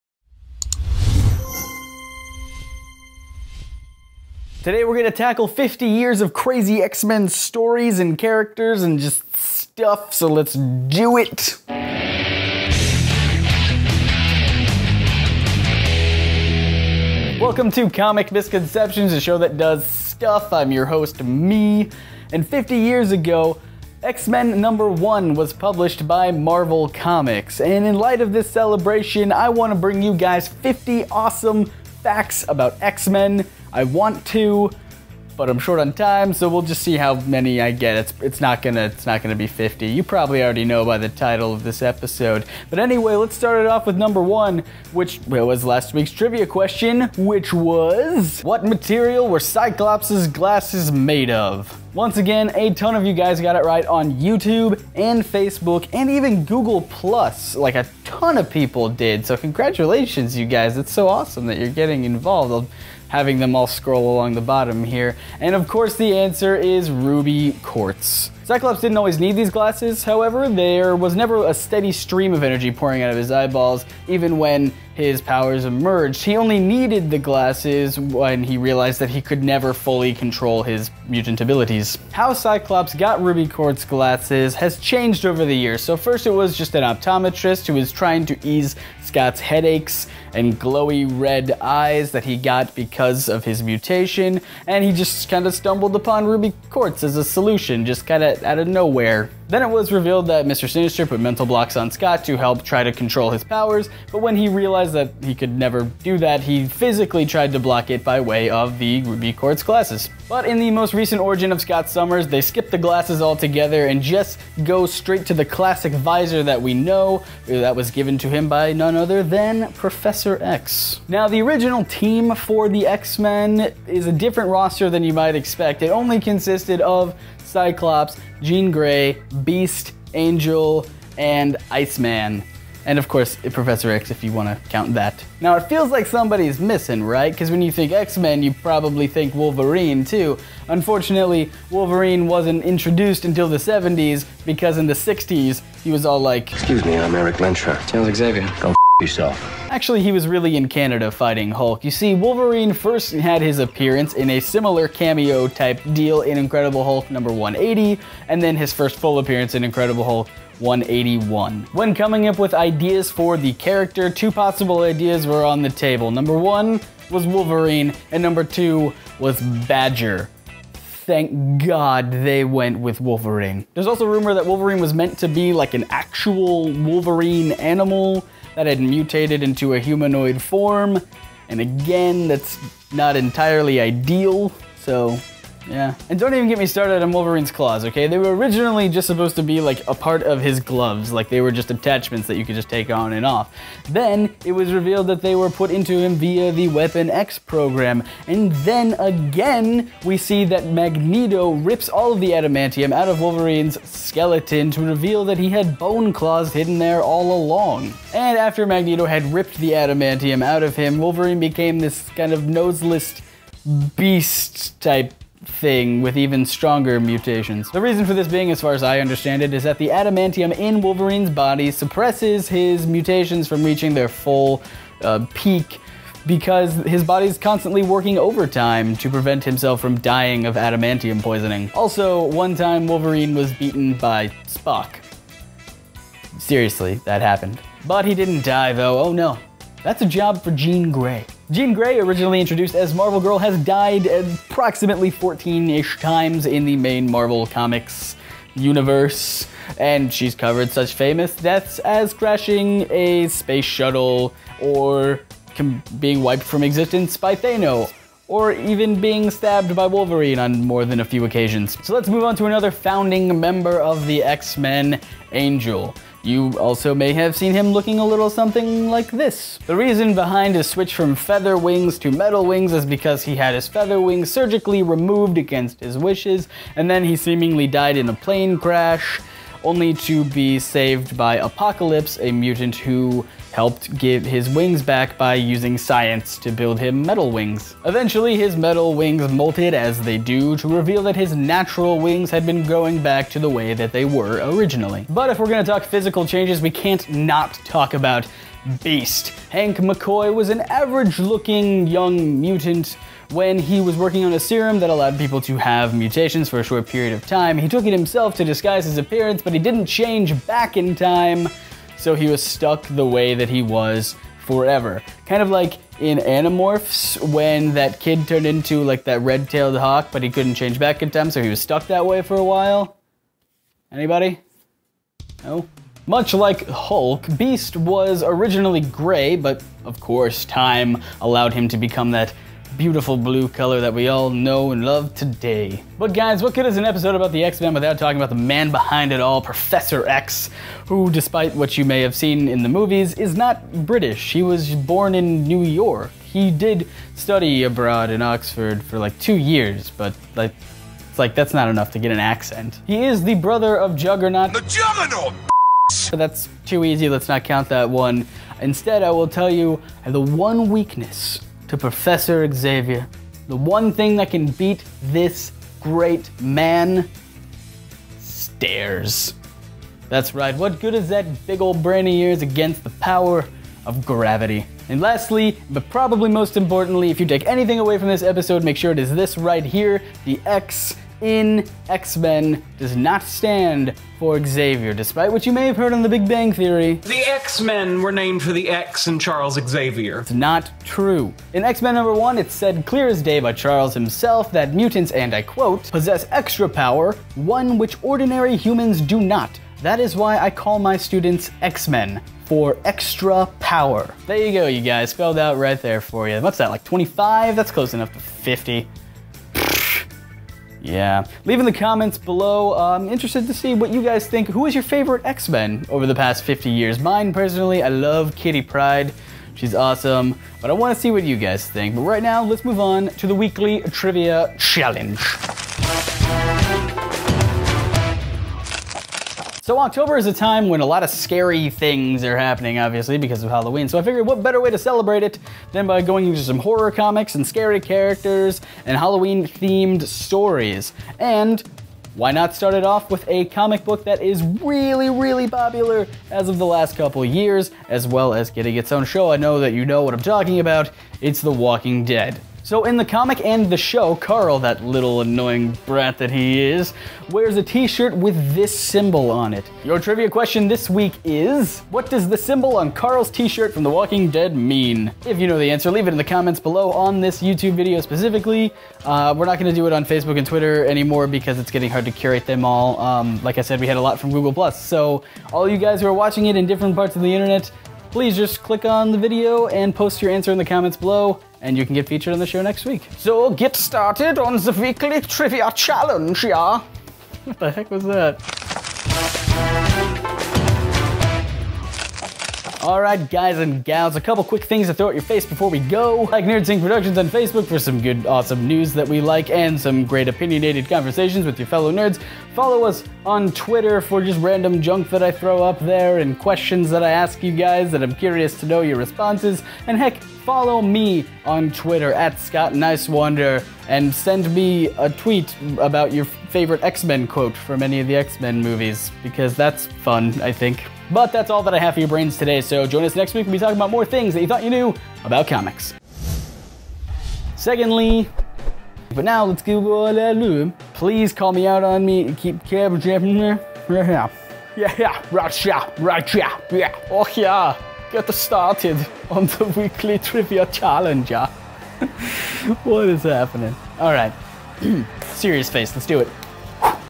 25 Uncanny Facts About the X-MEN! || Comic Misconceptions || NerdSync
Scott runs through 25 interesting facts about the X-Men in celebration of their 50th anniversary! Where was Wolverine originally introduced? How many times has Jean Grey died? What does the "X" in "X-Men" REALLY stand for? All that and more!

Every Wednesday, Comic Misconceptions explores fascinating trivia, crazy stories, and mind-blowing theories about the comic book universe from Marvel, DC, and beyond!